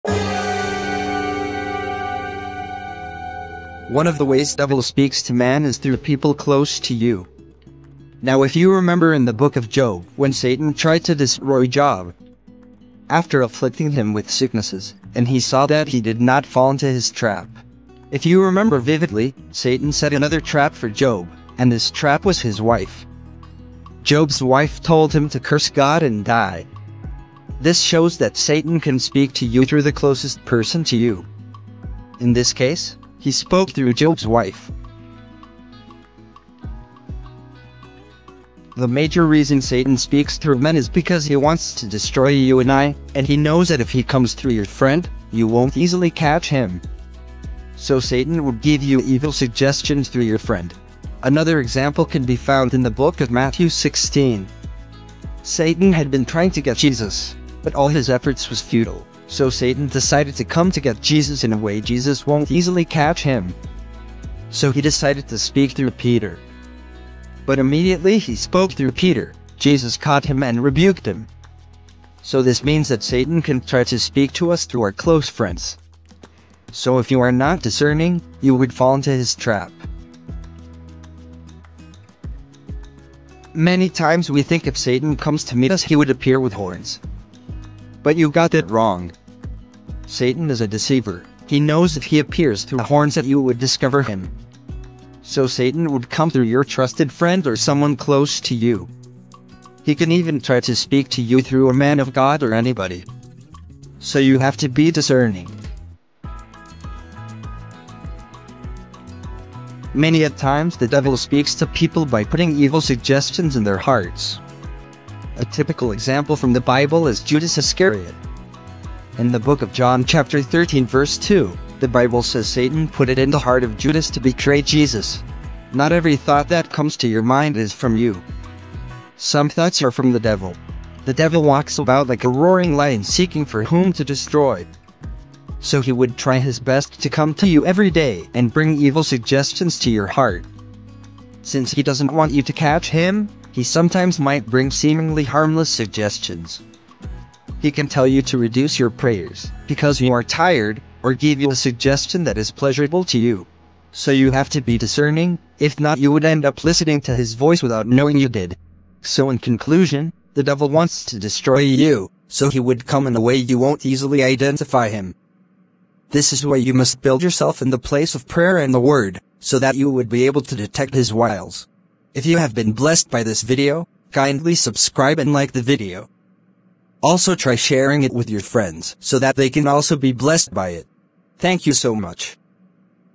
2 ways the Devil talks to you, that you don't know
The Devil is walking around like a roaring lion seeking whom to devour
The Devil wants to destroy mankind, and he does that by speaking to them, and giving them ungodly suggestions. But most times because we are ignorant of the devices of the Devil, he takes advantage of us.
In this video, I would be showing you 2 ways the Devil speaks to you that you don't know.
Ensure to watch till the end